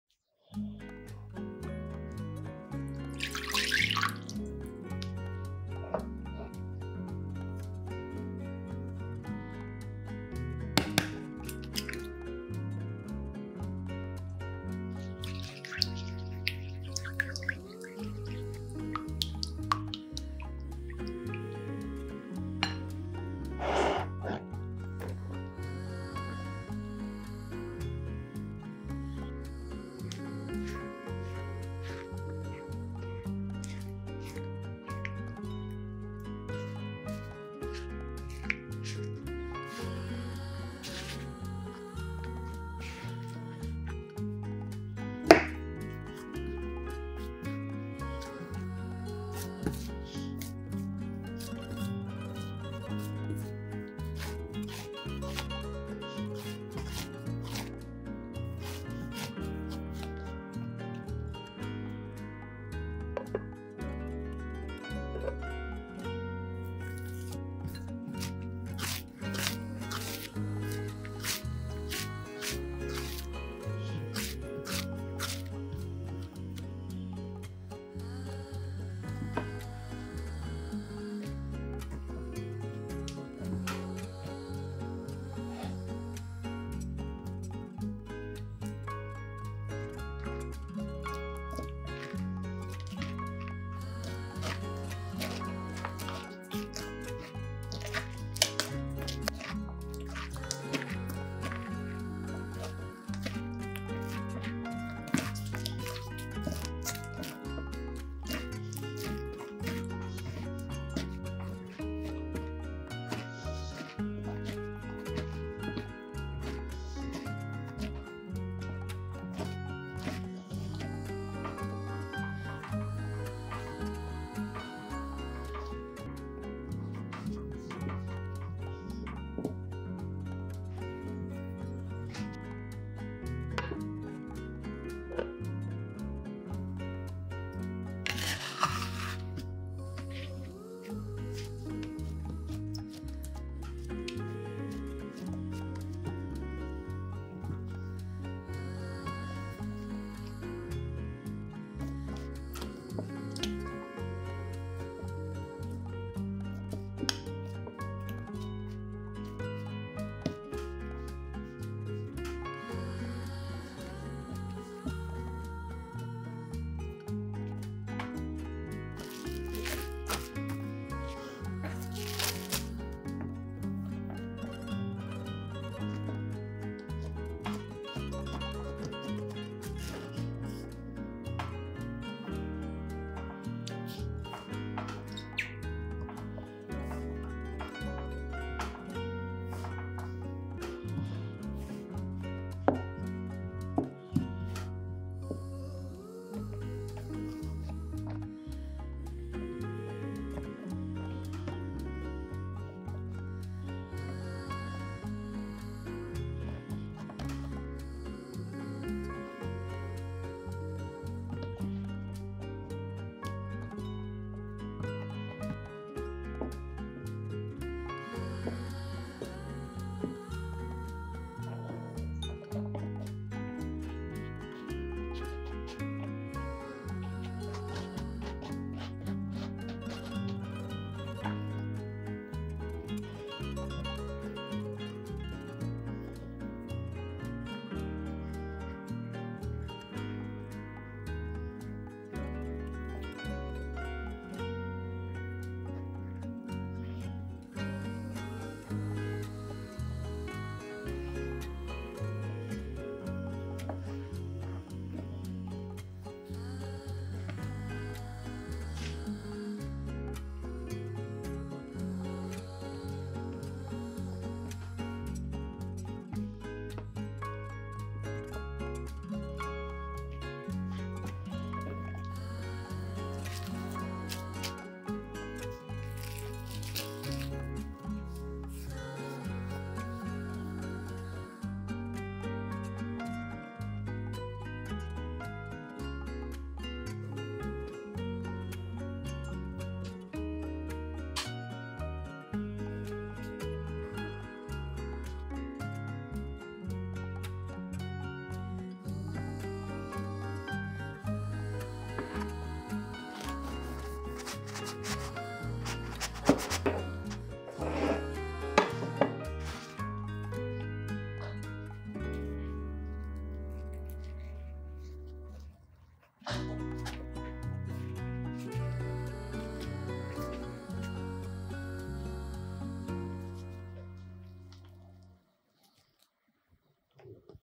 New bread recipe learned from my neighbor! Incredibly easy and delicius!
New bread recipe learned from my neighbor! Incredibly easy and delicious!
📢 Recipe (Ingredients)
300g (2 cups)    Bread flour/wheat flour
1    Egg
140g (0.6 cup)    Milk
3g (1 tsp)    Yeast
3g (1/2 tsp)    Salt
30g (2 tbsp)    Unsalted butter
Meat floss

* 175°C (350°F)  bake for 18 minutes